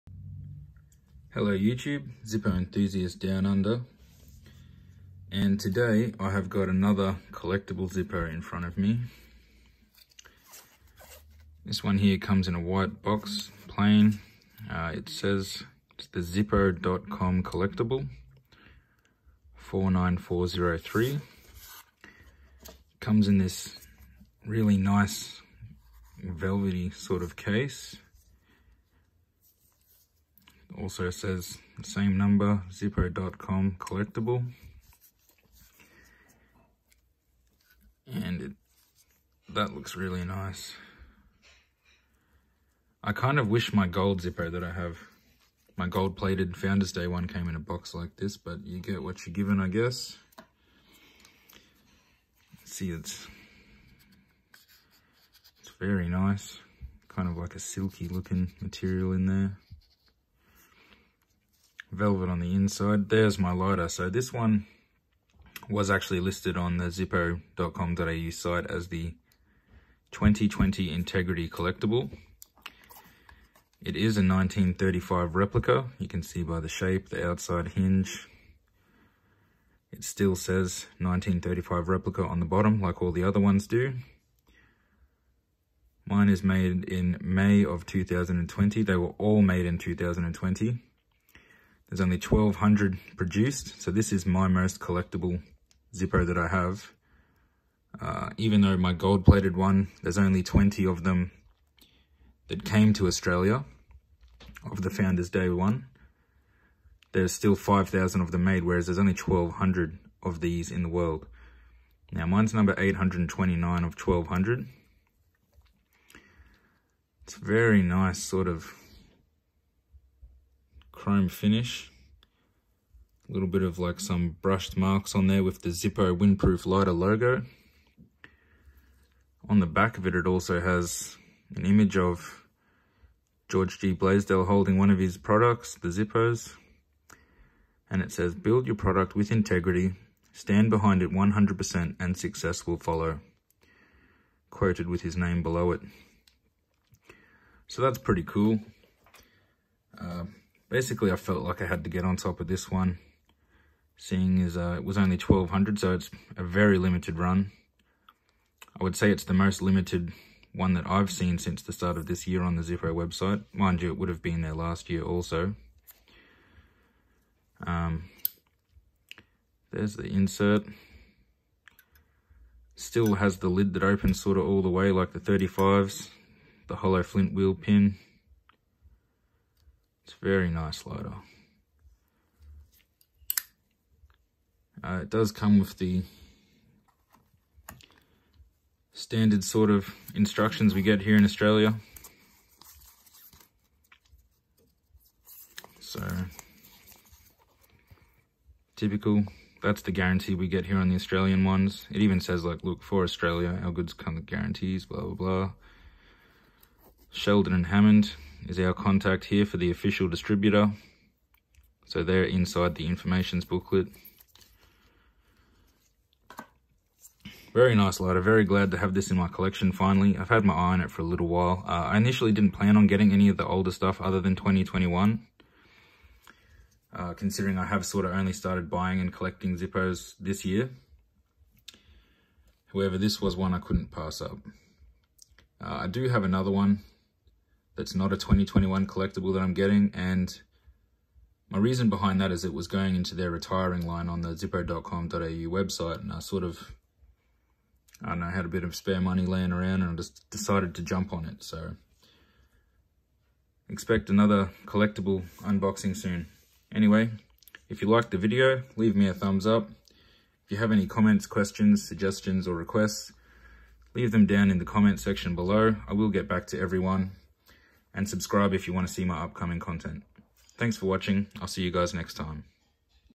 Unboxing Zippo Collectable - 2020 Integrity Collectable 1935 Replica
Quick unboxing showcasing the 2020 Integrity Collectible 1935 Replica Zippo.